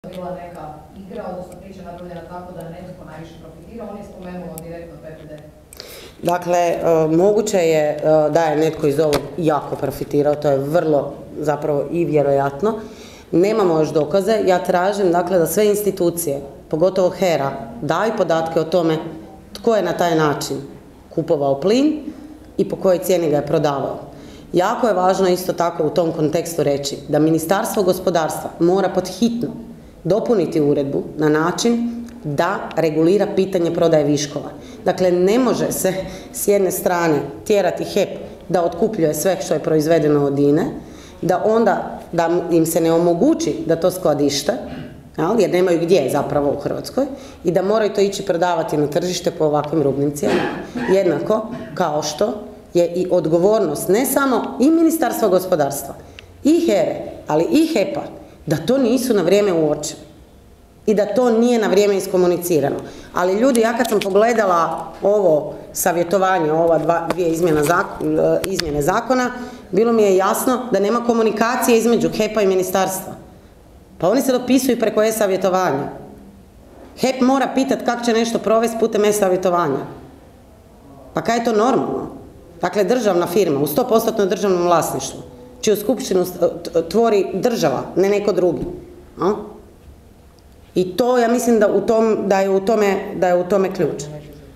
Benčić o aferi HEP: "Moguće je da je netko jako profitirao"
Na konferenciji za medije održanoj u Saboru o temi "Odgovara li novi zakonski okvir na potrebe za solarizacijom Hrvatske i potporom građanskoj energetici?" govorila je Sandra Benčić.